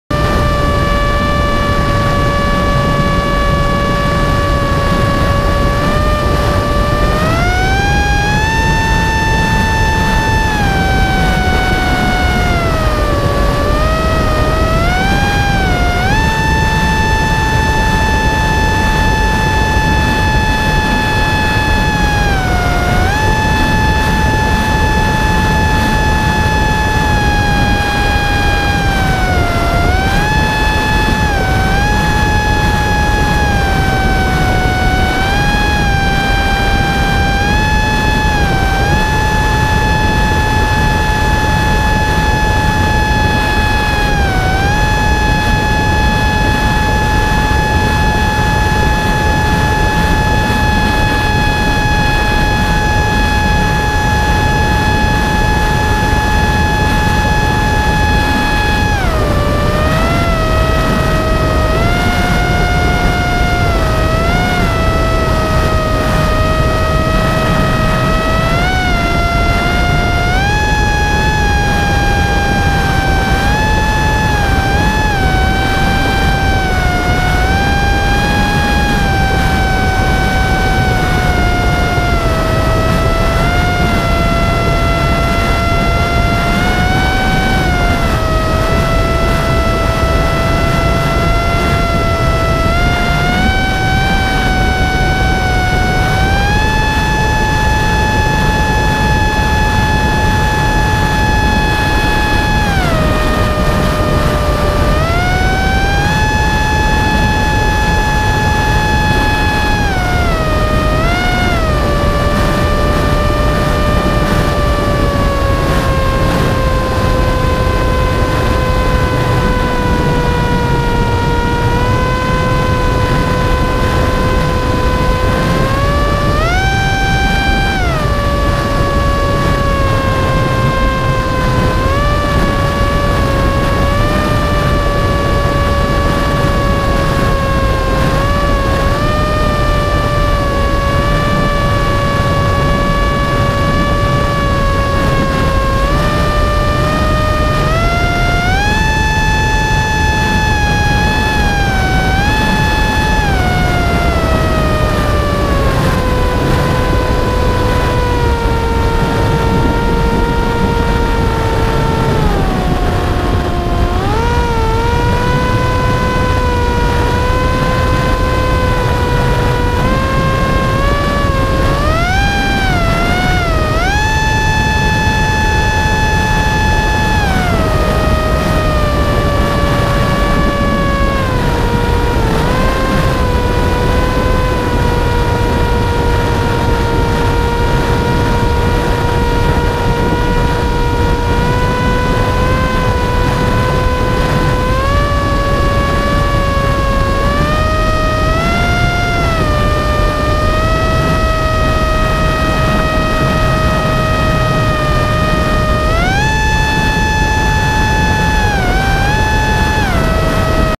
The Chase
Some raw footage of Abel and myself running around the park.  Slowing and speeding up to capture some footage.  I need more practice!